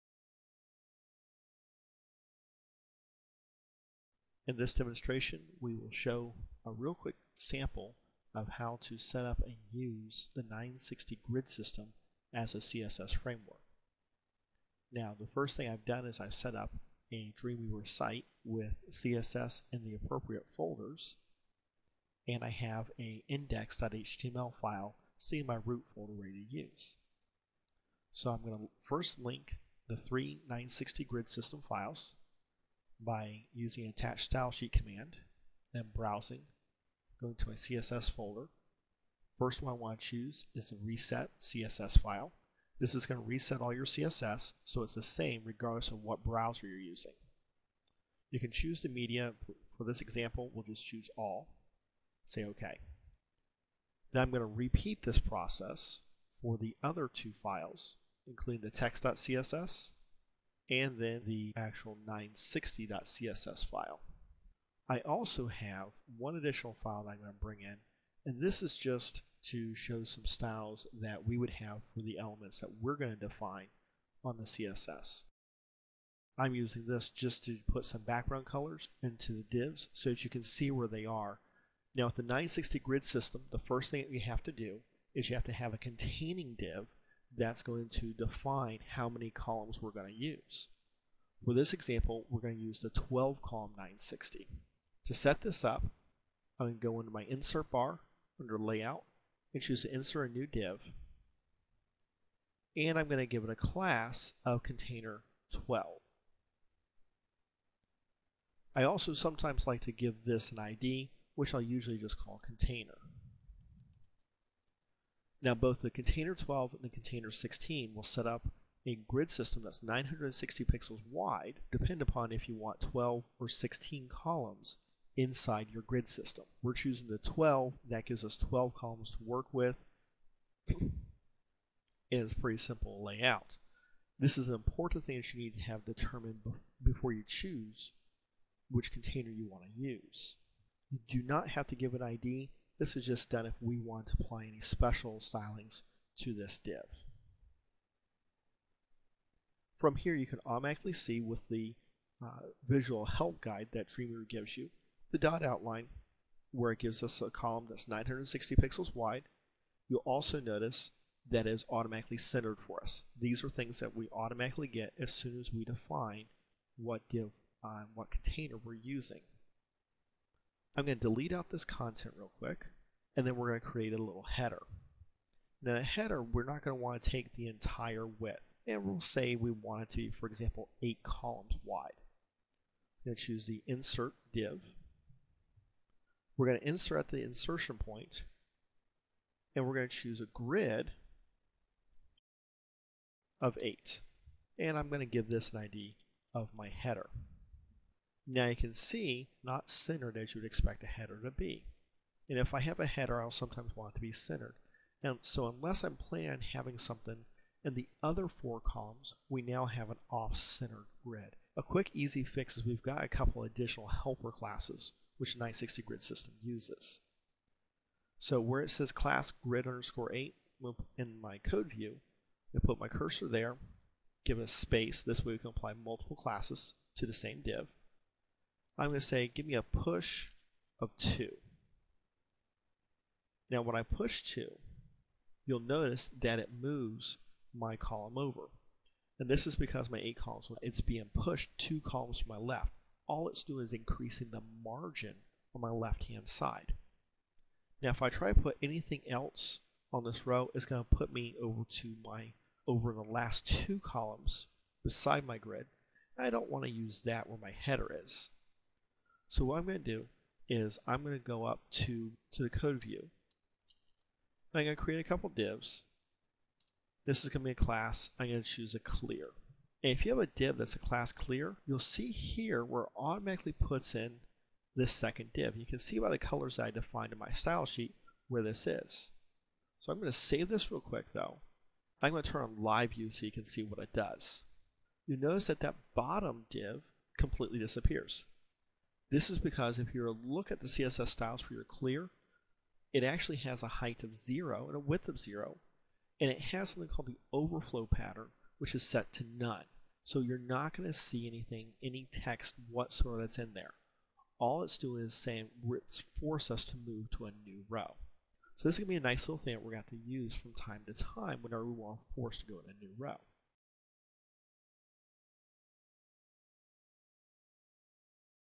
Set Up a 960 Grid System web page - Part 1
A Higher Quality version of the previous upload. How to set up a simple web page using the 960 grid system CSS framework. This video shows the initial set up, including the CSS files (in the correct order) adding a container div and centering a header which doesn't take the entire width of the page.